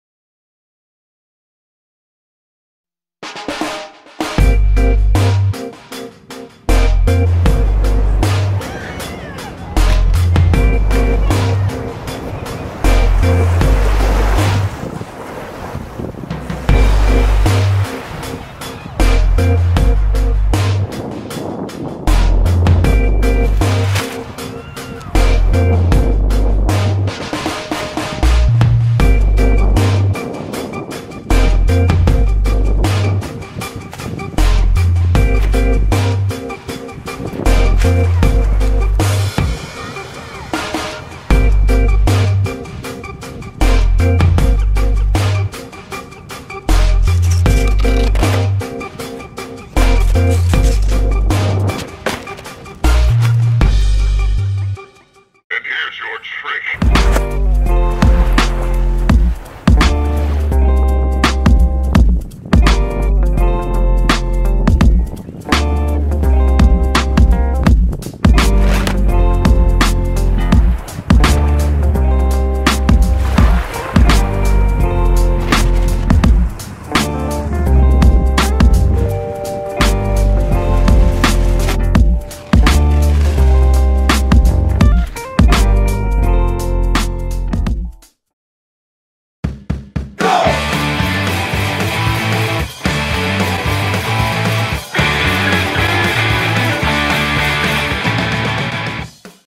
21:9 UHD - Urban Kiting - Rev Quad Line - Tricks
Flying Revolution Kite at beach doing tricks catches and throws filmed an Ultra hd anamorphic